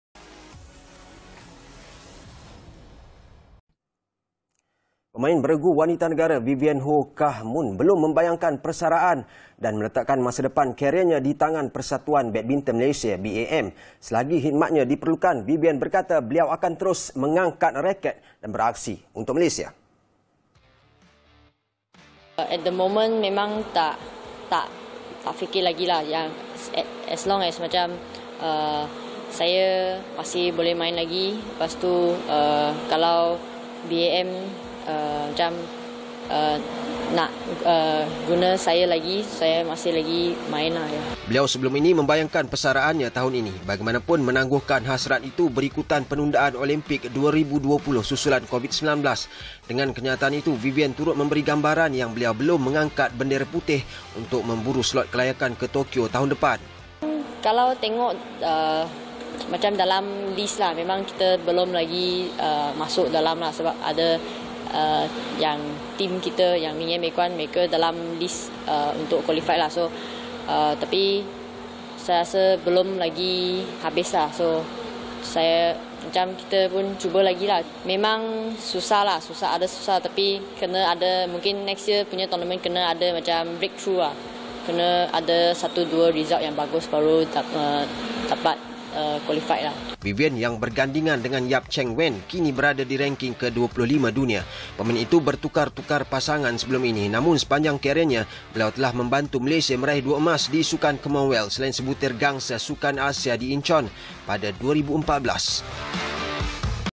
Vivian belum angkat bendera putih
Pemain Beregu wanita negara, Vivian Hoo Kah Mun belum membayangkan persaraan, dan meletakkan masa depan kariernya di tangan Persatuan Badminton Malaysia (BAM).

Selagi khidmatnya diperlukan, Vivian berkata, beliau akan terus mengangkat raket dan beraksi untuk Malaysia.